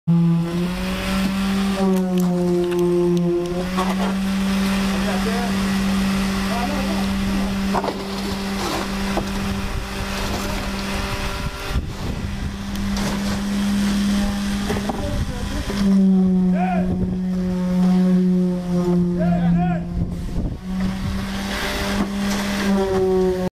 יציקת בטון בריכת גלישה
יציקת בטון בבריכה עם תעלות גלישה בראשל"צ בפיקוח חברת נטף מערכות מים
 n-nataf.co.il